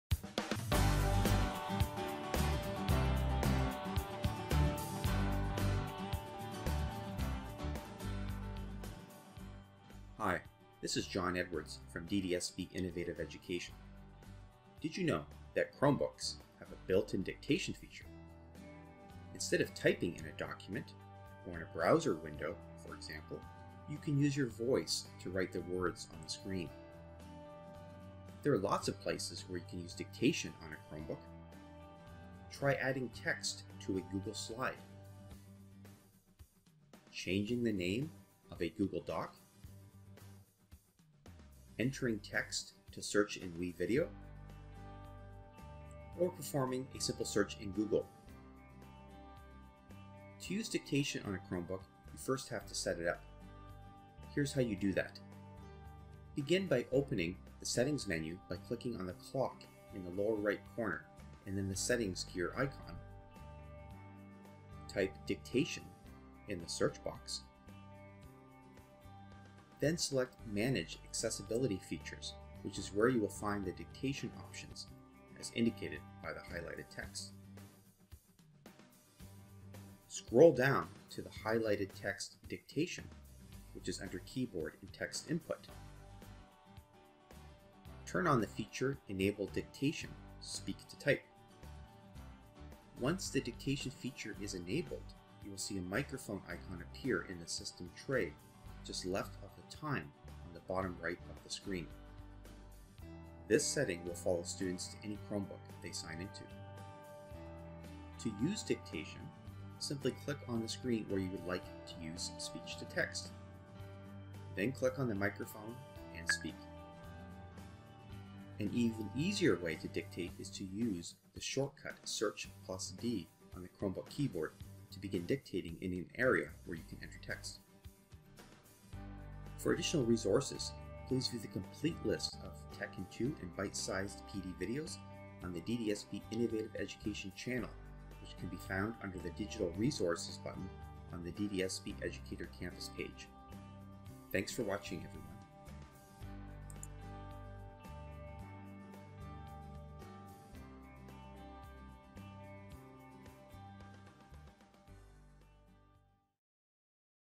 Chromebook Dictation Tech in 2
In this Tech in 2 we take a look at the dictation accessibility feature of Chrome OS on Chromebooks. This option allows students to use speech to text anywhere they can enter text.